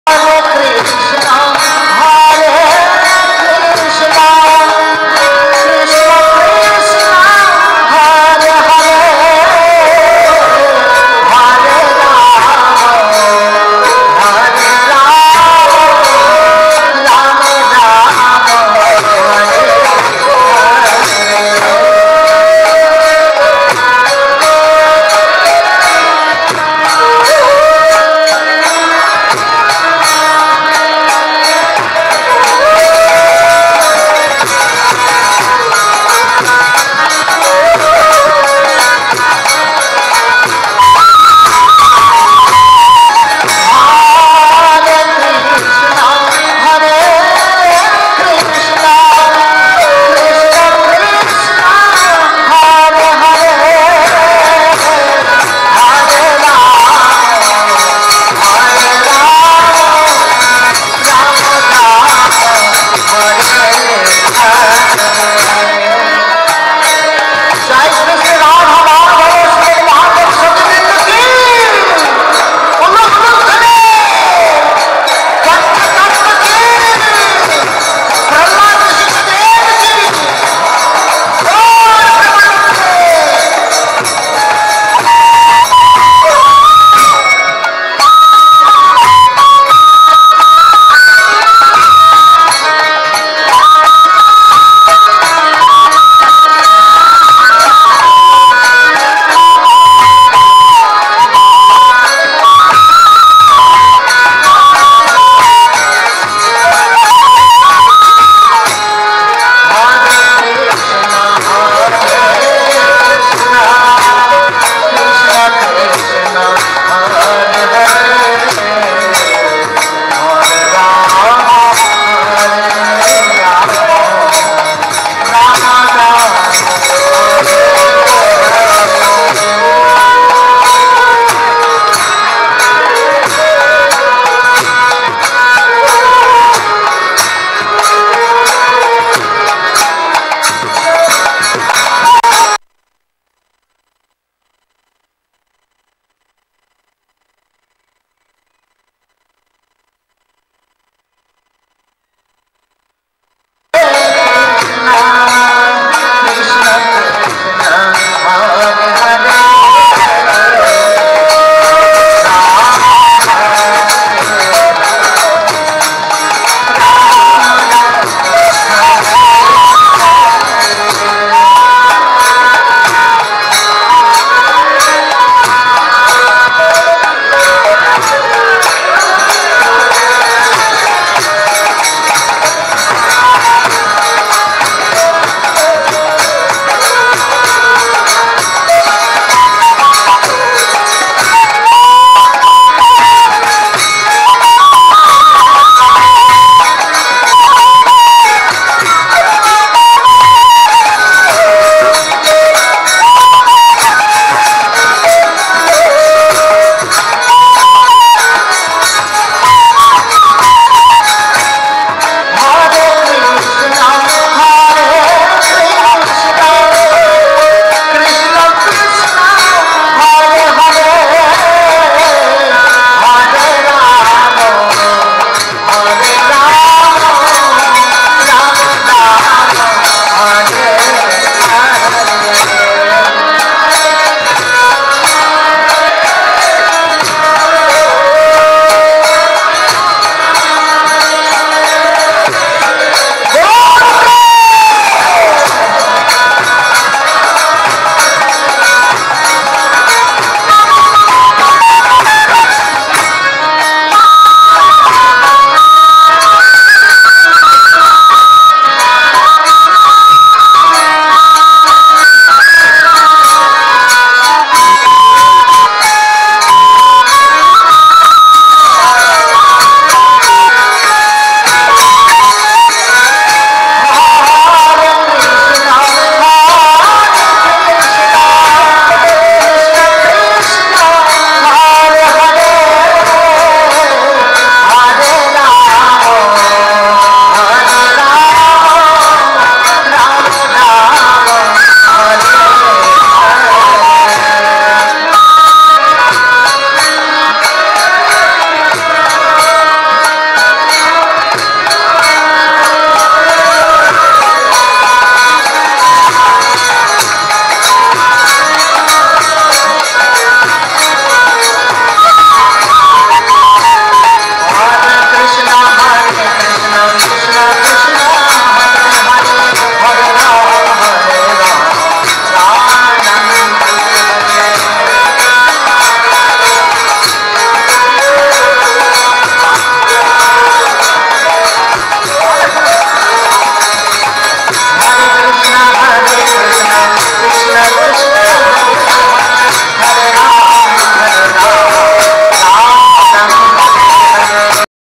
Rasayatra 2013, ISKCON,Mayapur
Arati Kirtan  during Rasayatra 2013 at ISKCON, Mayapur. Watch it and share your view.